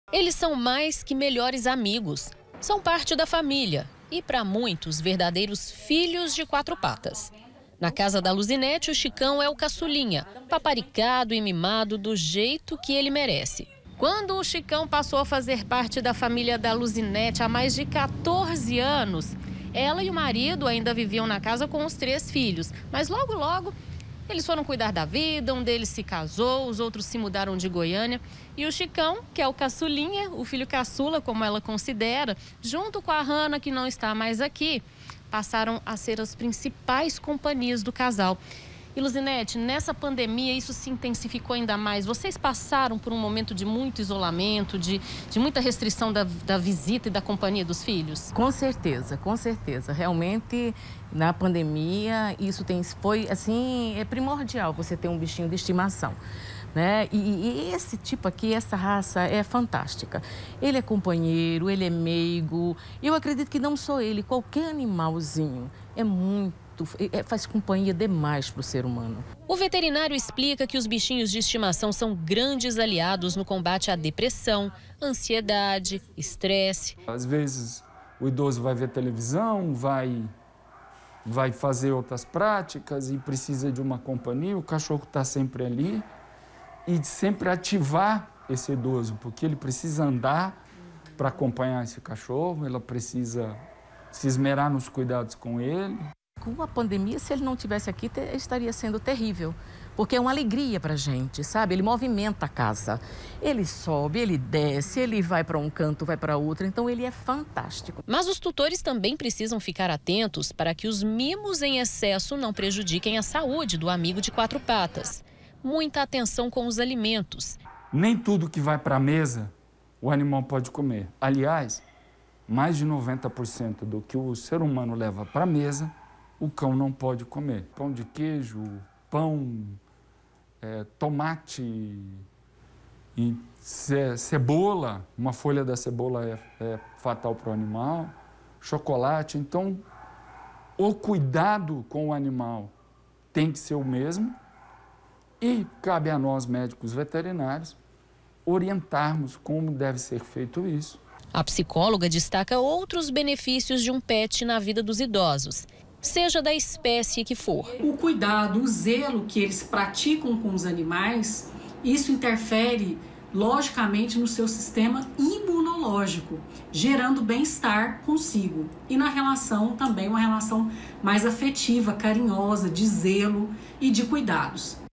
ANIMAIS DE ESTIMAÇÃO: BENEFÍCIOS PRA SAÚDE FÍSICA E MENTAL DOS TUTORES
NINGUÉM MAIS DUVIDA DOS BENEFÍCIOS DA RELAÇÃO ENTRE HUMANOS E ANIMAIS. E NESTA PANDEMIA, OS PETS TEM FEITO TODA A DIFERENÇA NA ROTINA DAS PESSOAS, PRINCIPALMENTE DOS IDOSOS.